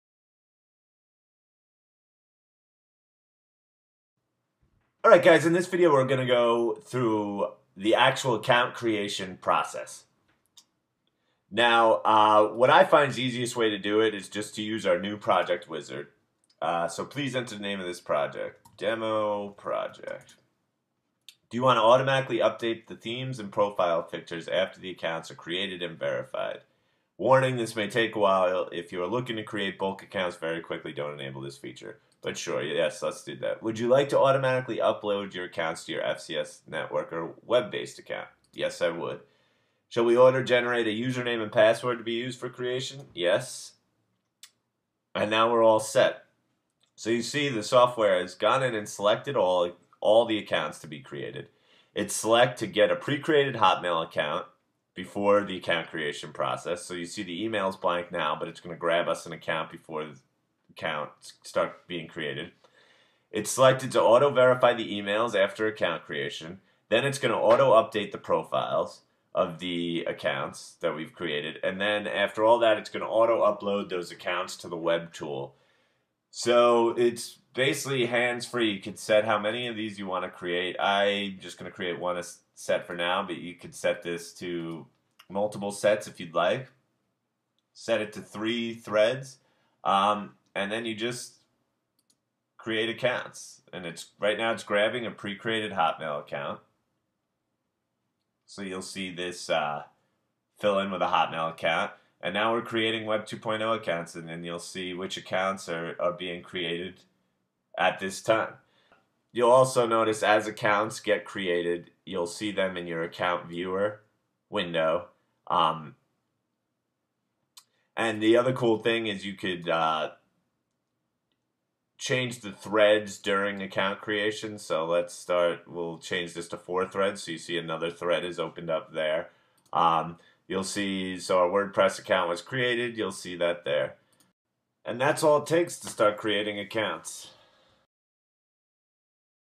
FCS Networker Quickly Creating Accounts
This shows how to quickly go about setting up an account creation project in the FCS Networker account creator software.